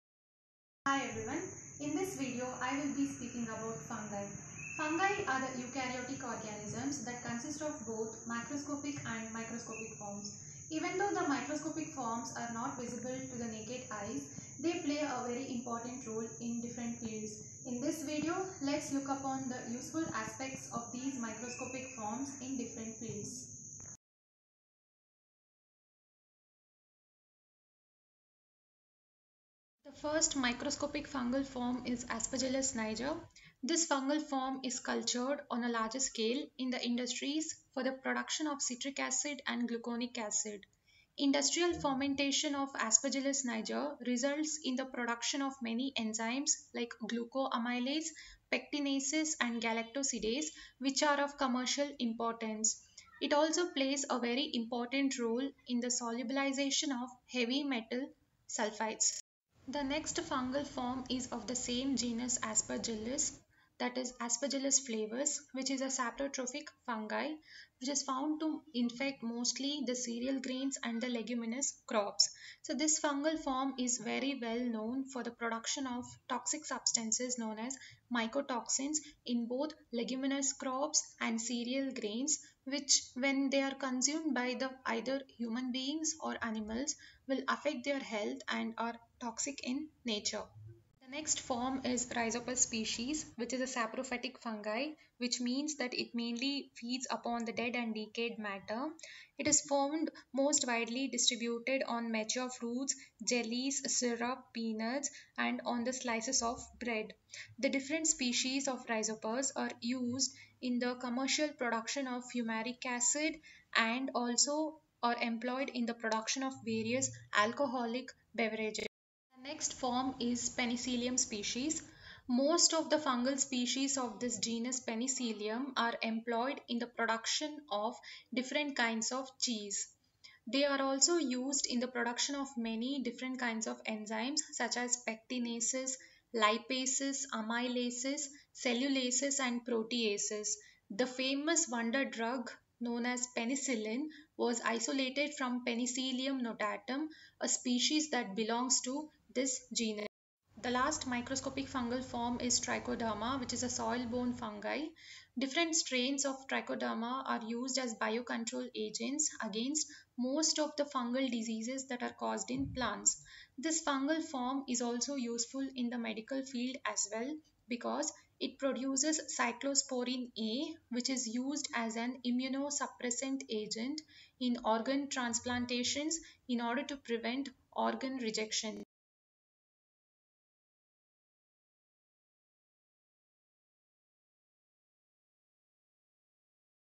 Microscopic fungii
This video consists of useful aspects of microscopic fungi such as Rhizopus, Aspergillus, Penicillium and Trichoderma genus. all the pictures of the fungi used in this video are clicked using the stereo binocular microscope and the fungal forms were cultured on different pulses.